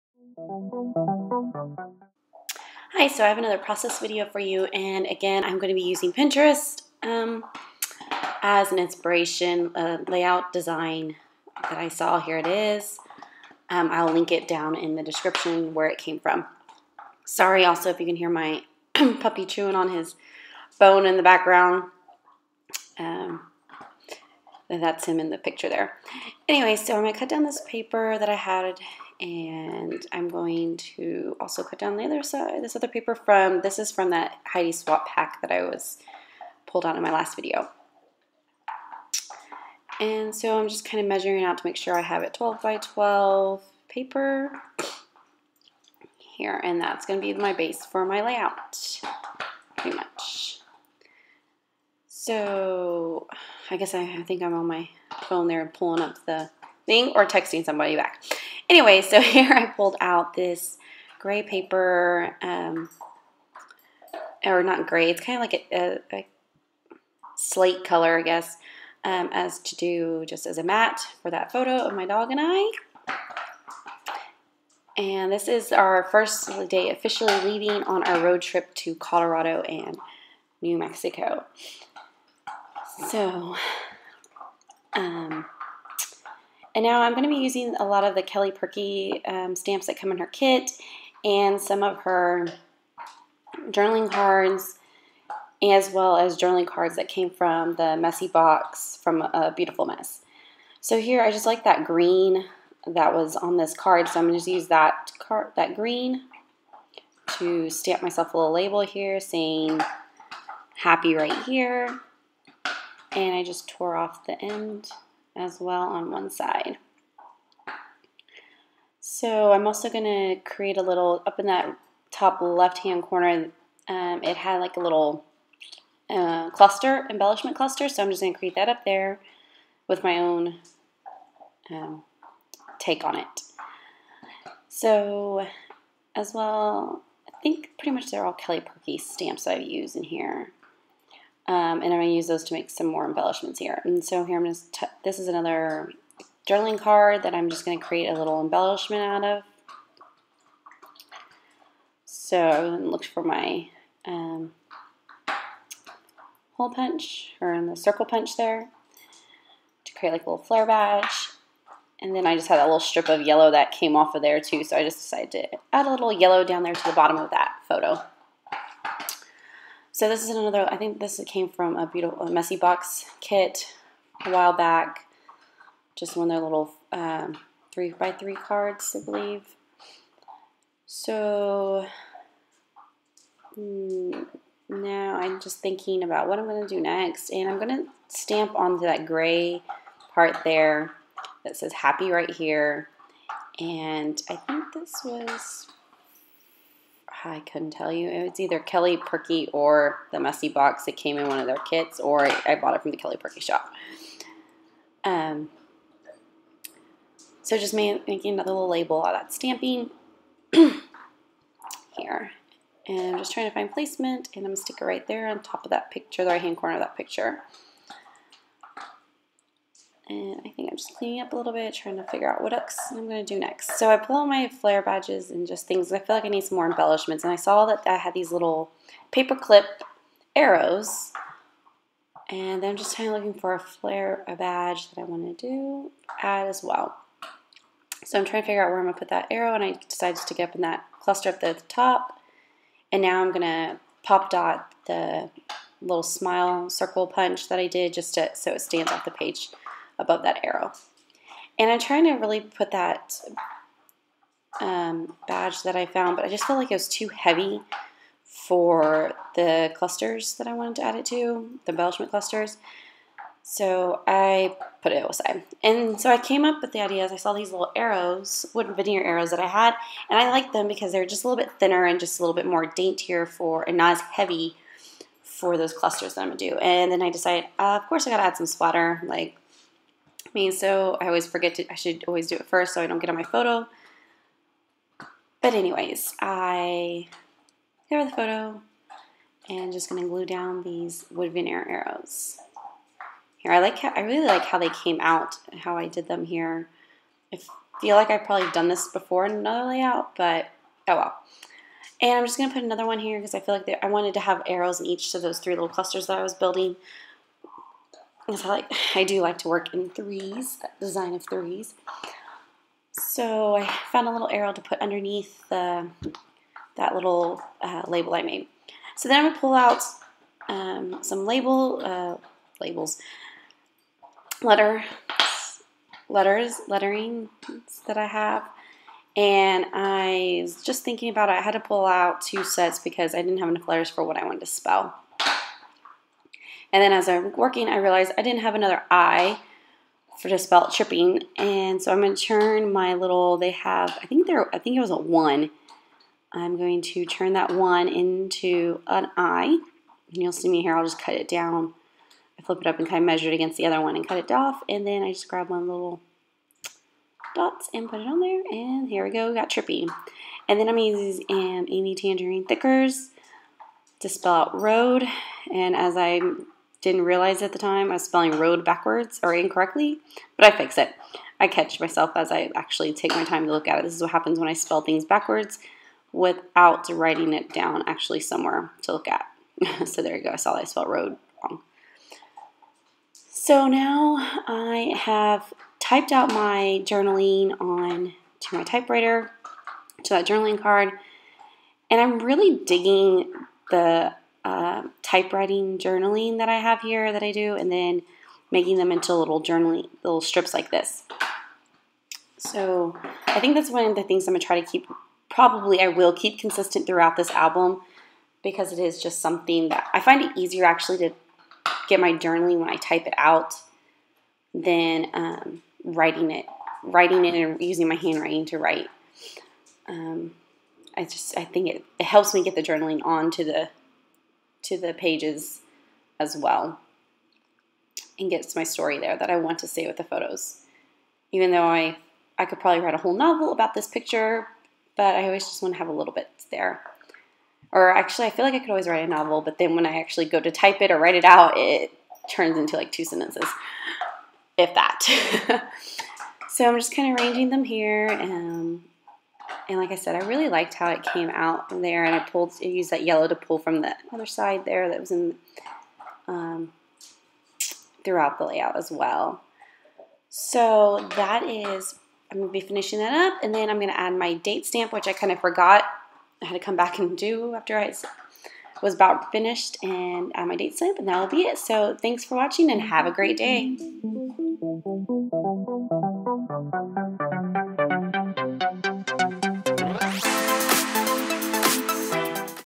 Road Trippin' Album
In this video, I scrap lift from a layout I found on Pinterest but adding my own style and emblishment changes. This is a 12x12 layout. Linked below is the layout.

Products | Materials

Heidi Swapp
Kelly Purkey
The Messy Box by Beautiful Mess